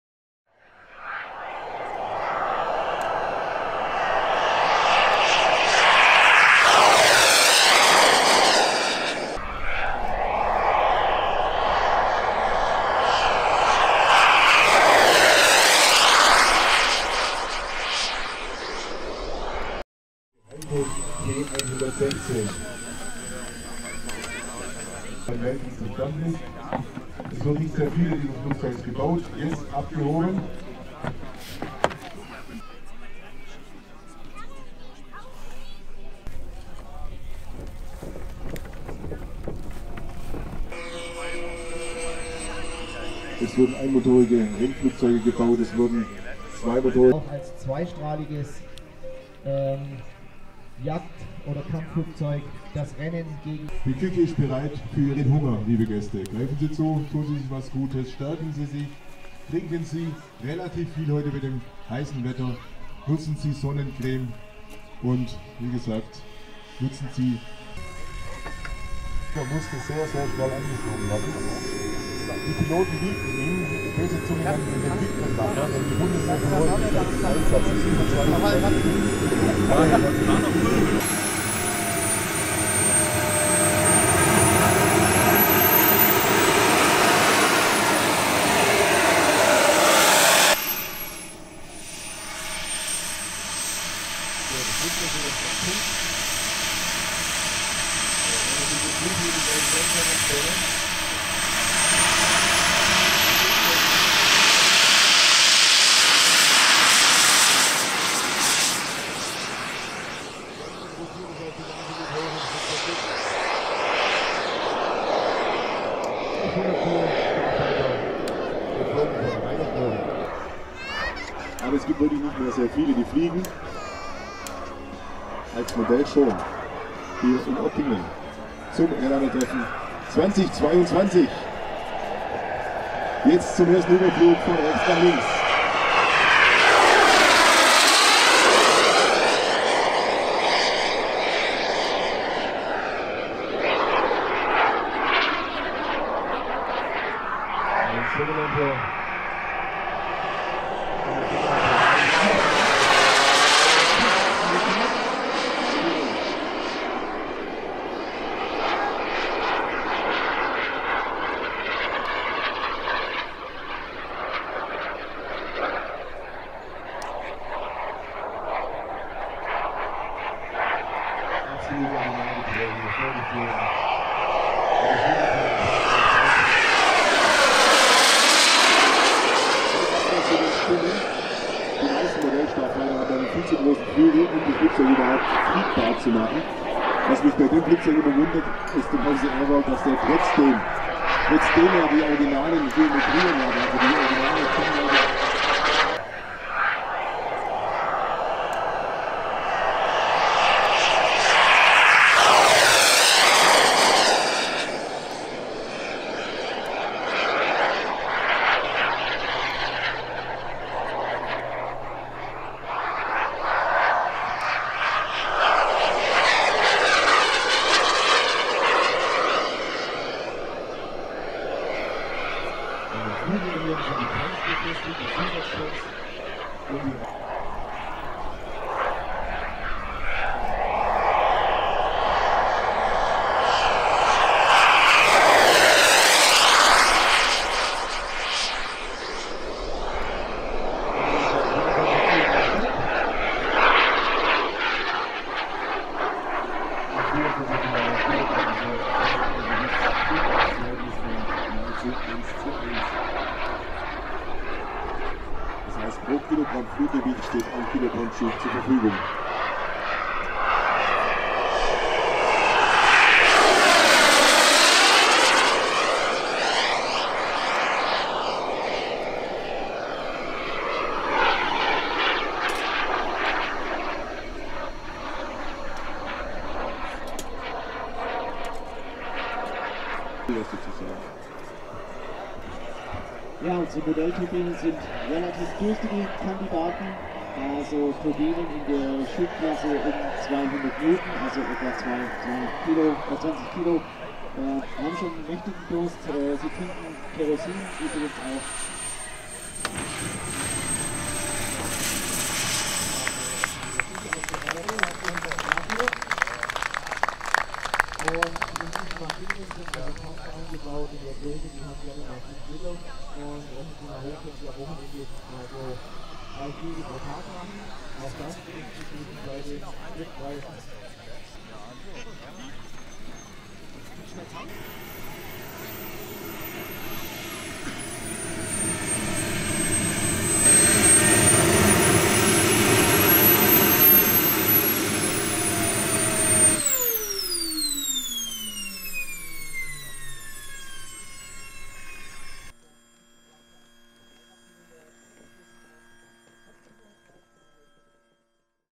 GIANT RC CF-104 Starfighter Turbine Jet Flight Demonstration, Scale 1:4
Pilot: Rainer Strobel
Model: F-104 Starfighter
Length: 4,17 m
Wing span: 1,82 m
Scale: 1:4
Weight: 24 kg

Event: Airlinertreffen 2022, Oppingen-Au